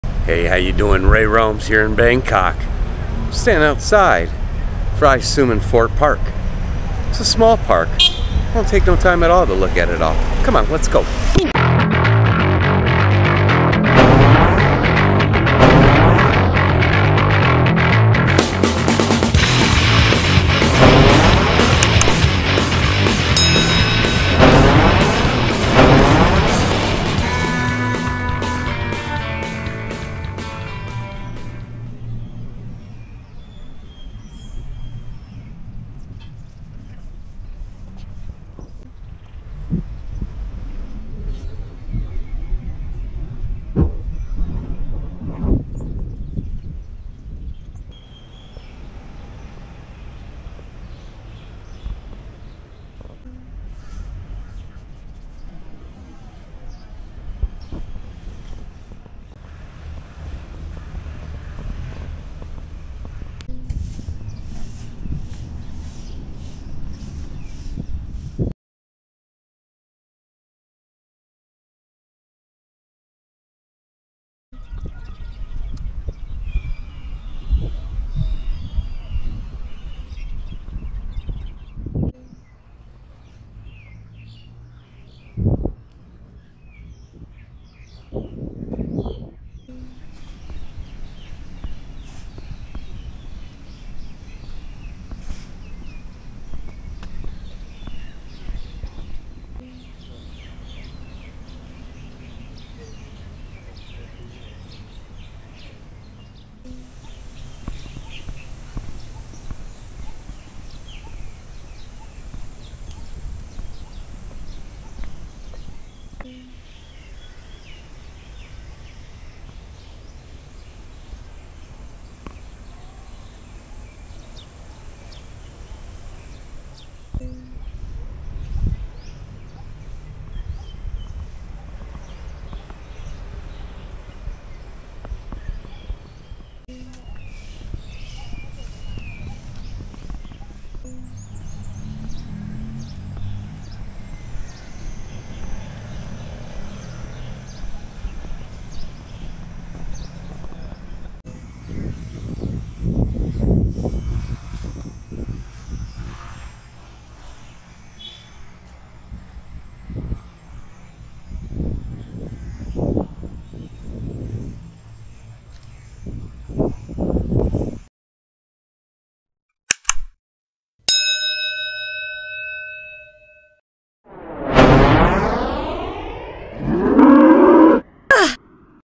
Ray Roams To Phra Sumen Fort Park | Bangkok | Thailand 🇹🇭
Ray roams to Phra Sumen Fort Park in Bangkok, Thailand 🇹🇭.

Santi Chai Prakan Park, located in the heart of Bangkok, Thailand, is a beautiful and historic green space that is beloved by locals and visitors alike. This tranquil oasis offers a respite from the hustle and bustle of the city and is a popular spot for picnics, strolls, and outdoor activities.

The park is named after Phra Santi Chai Prakan, a revered royal family member who played a significant role in the development of the park. It was originally established as a part of the royal palace grounds and was later opened to the public, becoming a public park in 1979. The park is steeped in history and features a variety of historical landmarks, including ancient city walls, fortifications, and the iconic Phra Sumen Fort.

One of the park's main attractions is the stunning Loha Prasat, also known as the Metal Castle. This unique and intricately designed structure is a representation of the original Loha Prasat found at Wat Ratchanadda, and it serves as a symbolic reminder of the rich cultural heritage of Thailand.

Santi Chai Prakan Park is also known for its lush greenery and beautiful gardens, making it an ideal place for nature lovers to relax and unwind. The park is home to a variety of trees, plants, and flowers, creating a peaceful and serene environment that provides a welcome escape from the urban landscape.

In addition to its natural beauty and historical significance, the park also hosts various events and activities throughout the year, including cultural festivals, art exhibitions, and live performances. Visitors can also enjoy recreational facilities such as jogging tracks, exercise equipment, and playgrounds, making it a great place for families and individuals to enjoy outdoor activities.

Overall, Santi Chai Prakan Park is a must-visit destination for anyone looking to experience the rich history, natural beauty, and cultural heritage of Bangkok. Whether it's taking a stroll, exploring the historical landmarks, or simply relaxing in the tranquil surroundings, this park offers a unique and memorable experience for all who visit.

Gear I Utilize
iPhone 13 Pro
RODE Mic
Canon 70D
Canon 50mm